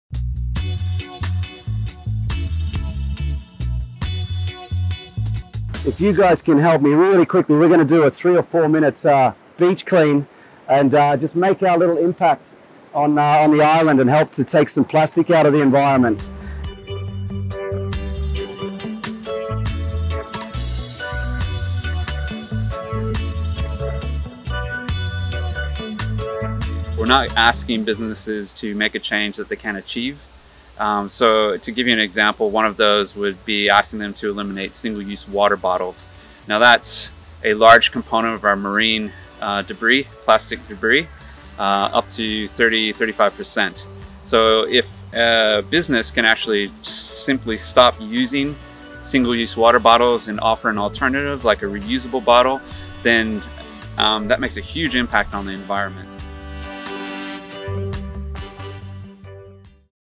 Solidarity in Tourism
Besides enjoying the activities in K'gari - Fraser Island, tourists help Drop Bear's crew taking some plastic out of the environment.